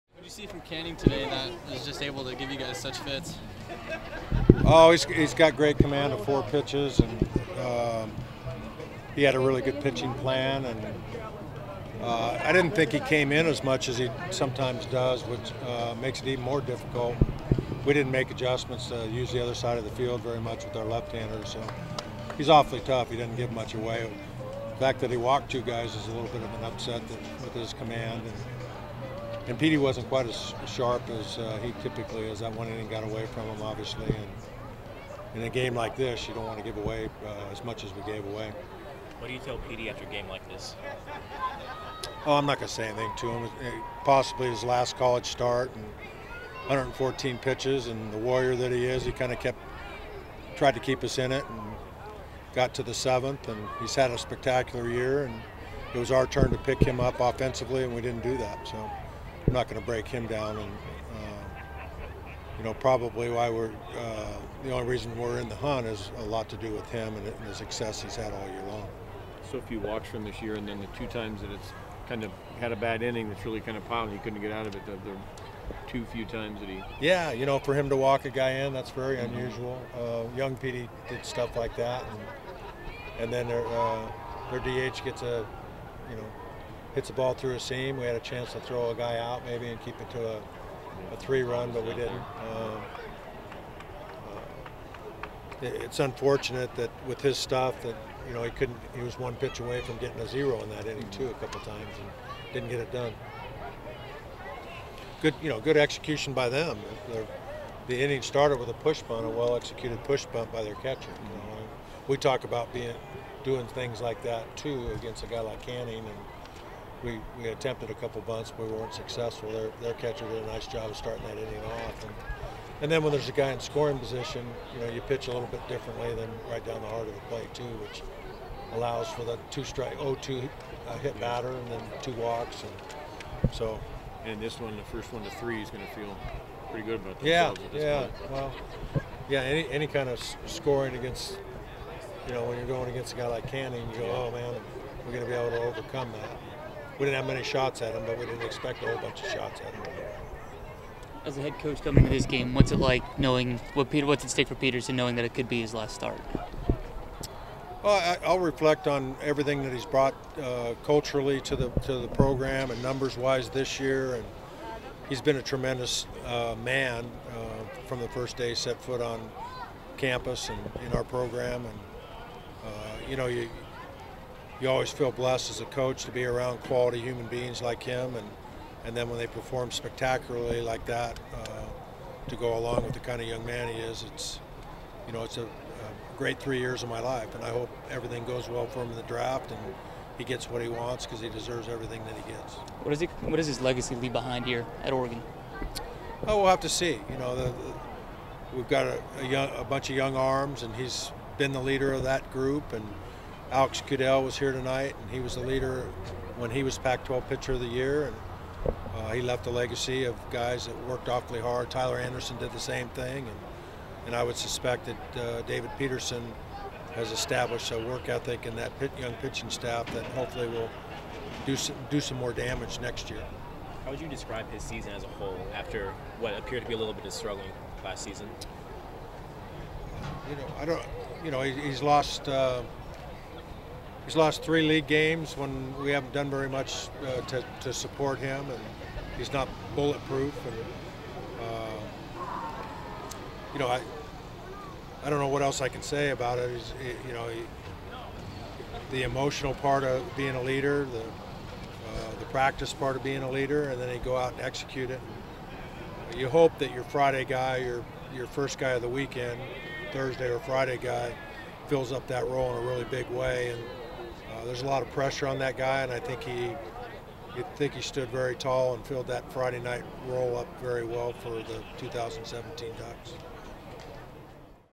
George Horton Following Series-Opening Loss to UCLA
5-25